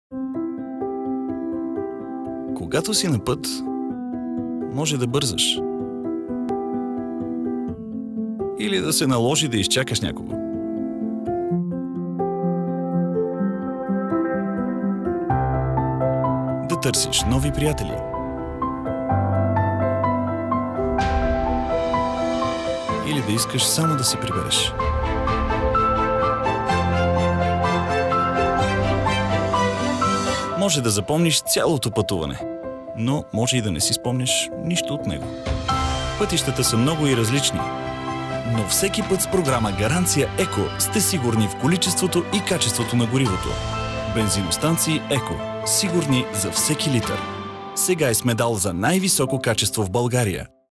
Реклама ЕКО Гаранция
Заснемане на височинни кадри с дрон за телевизионна реклама на Бензиностанции ЕКО
Дата: април 2017 г.
Клиент: Screen Brothers, ЕКО България